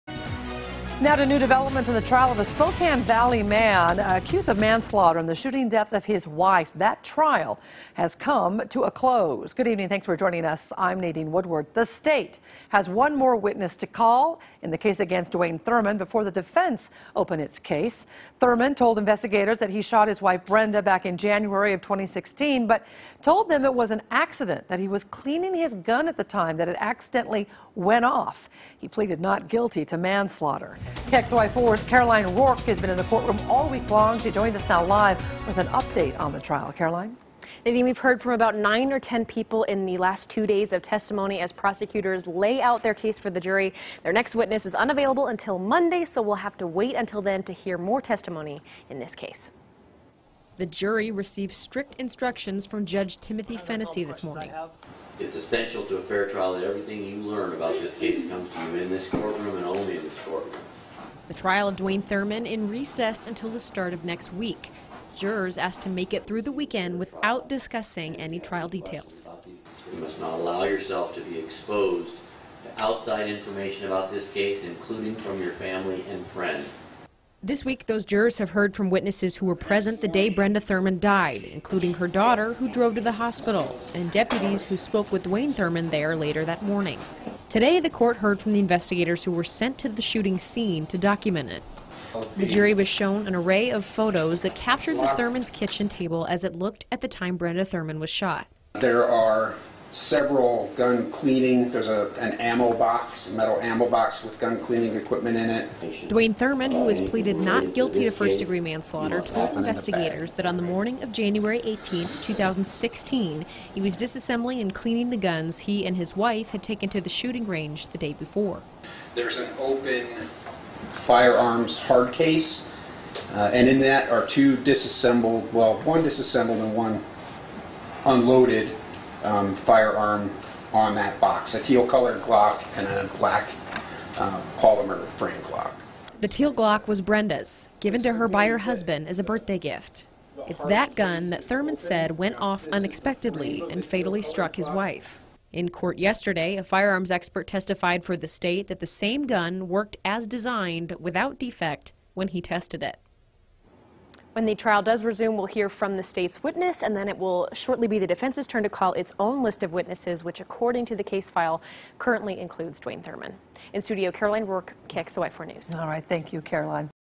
Thurman trial in recess until Monday
Dwayne Thurman has pleaded not guilty to a manslaughter charge stemming from the shooting death of his wife in 2016. KXLY4's Caroline Rourke reports.